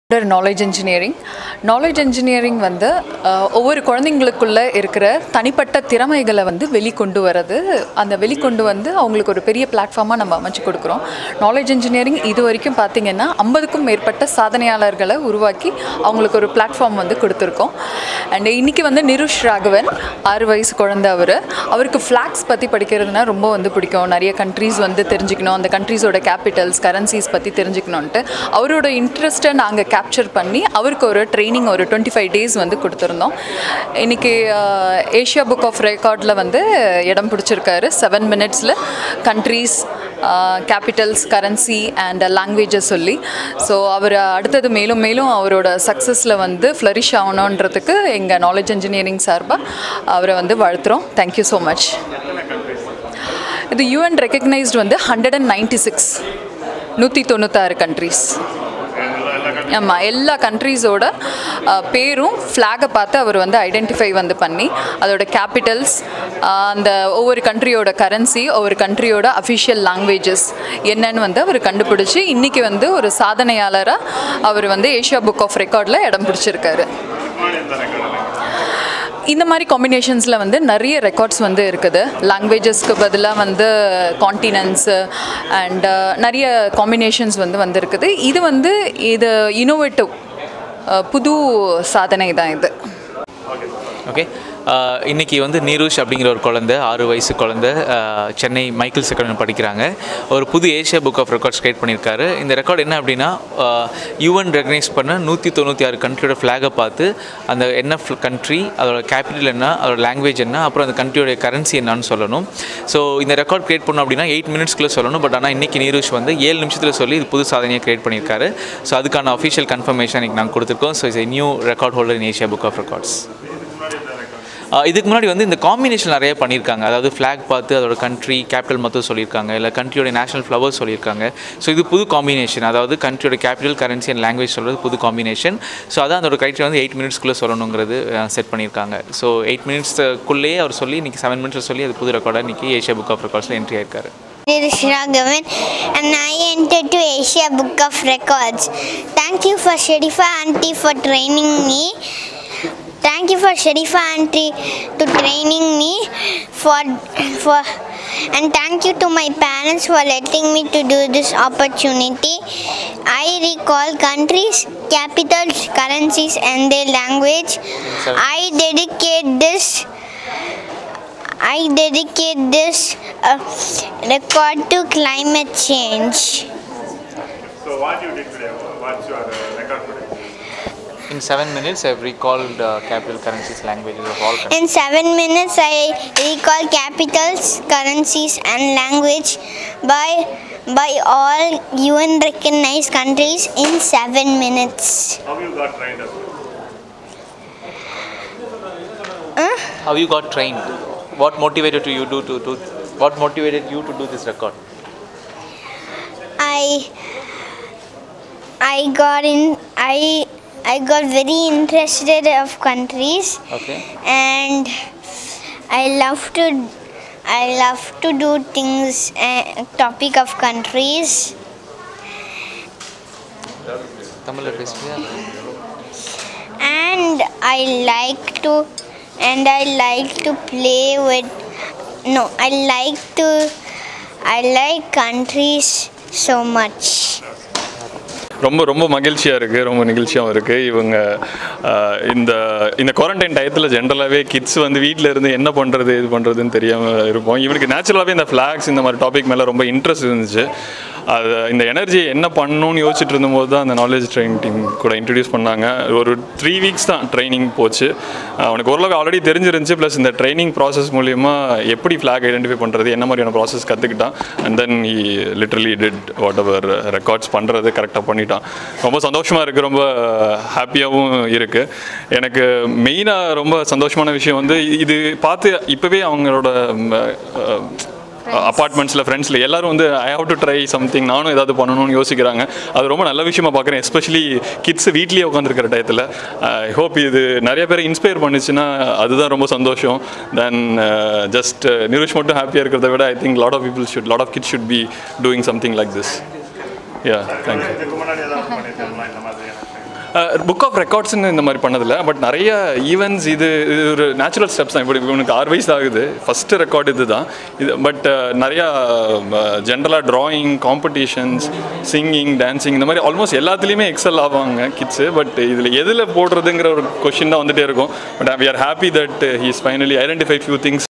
சிறுவனின் திறமையை பார்த்து வியந்து போன நடுவர்
உலக சாதனை படைத்த 6 வயது சிறுவன்

FASTEST TO RECALL CAPITALS, CURRENCIES AND LANGUAGES OF ALL COUNTRIES BY THEIR FLAGS